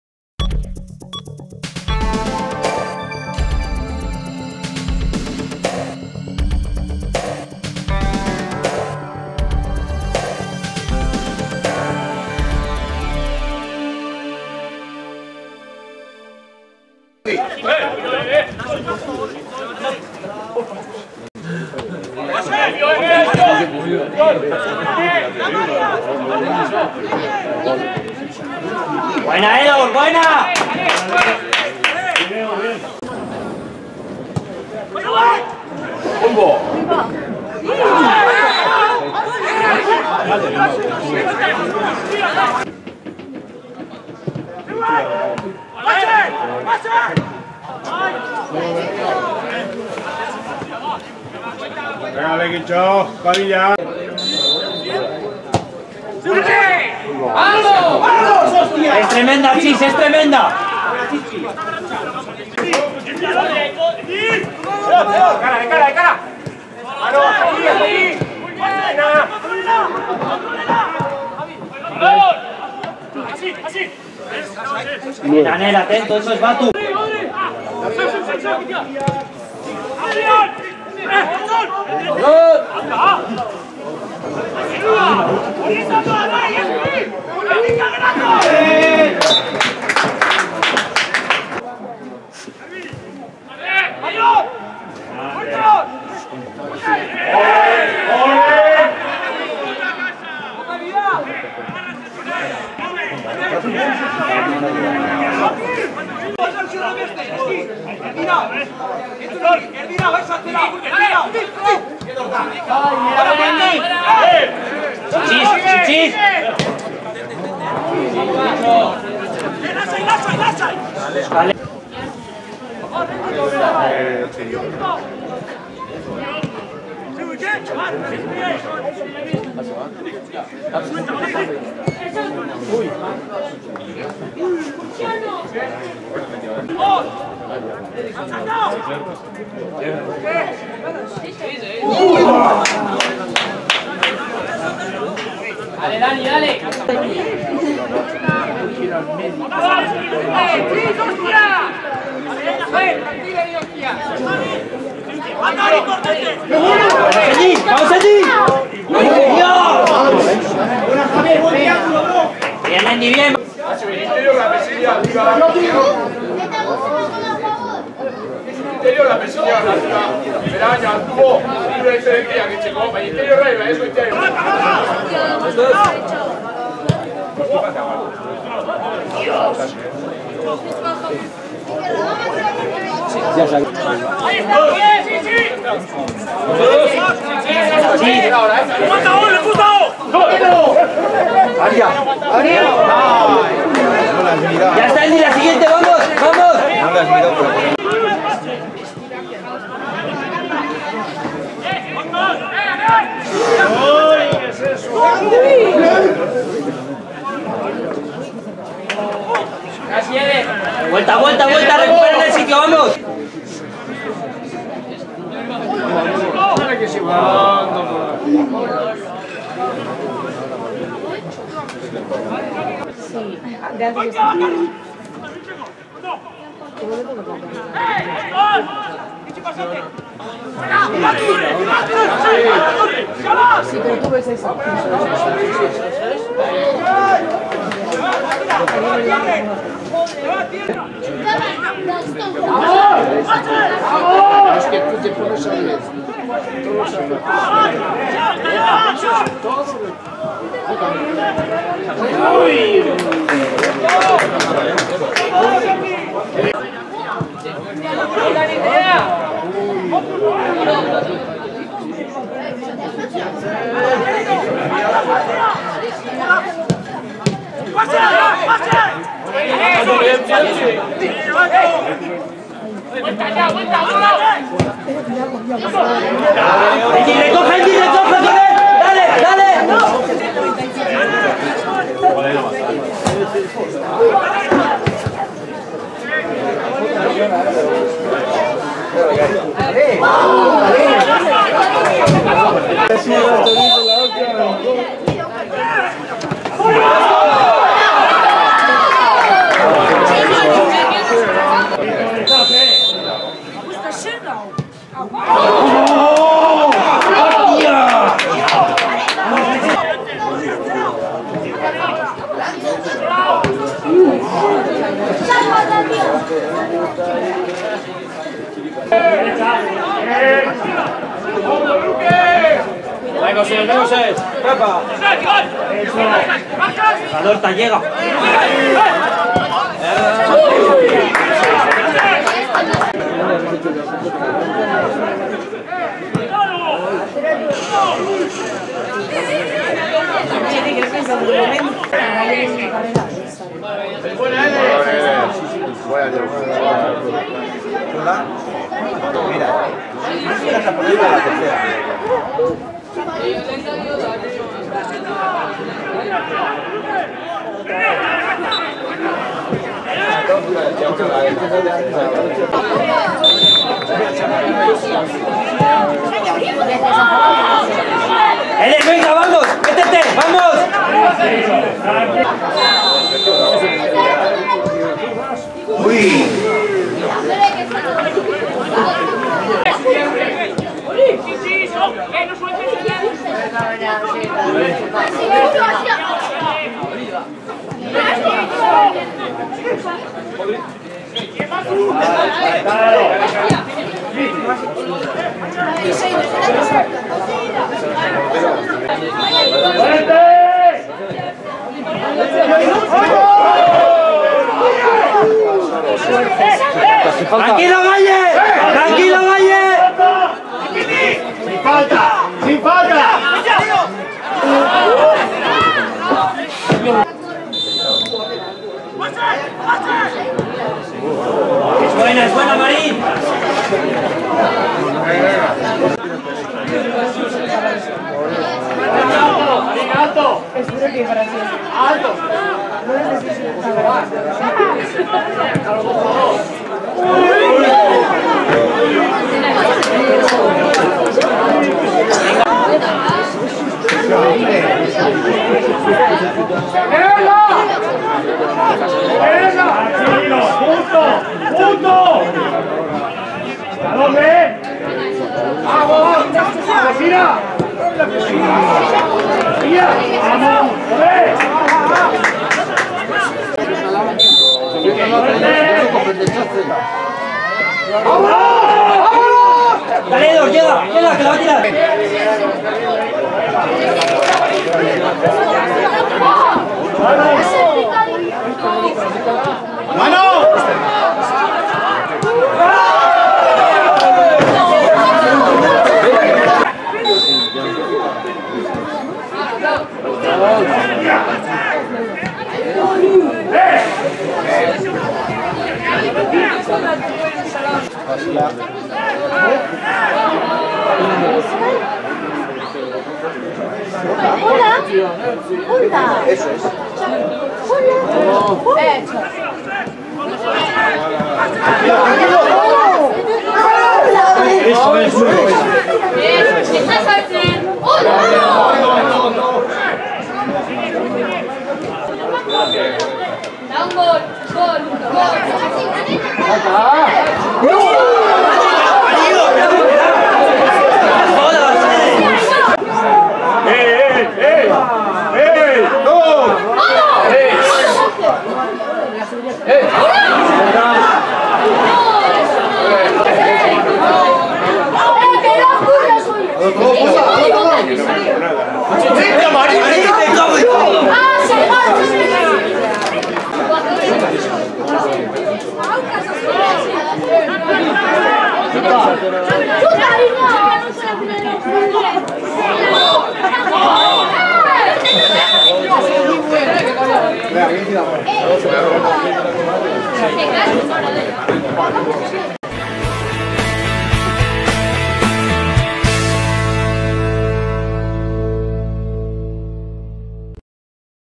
LEKEITIO F.T. 0:0 C.D. Basconia B (Kepa Naberan, 2023/03/05)
2023ko Martxoaren 5ean Lekeitioko Kepa Naberan futbol zelaian jokatu zen partiduaren laburpena.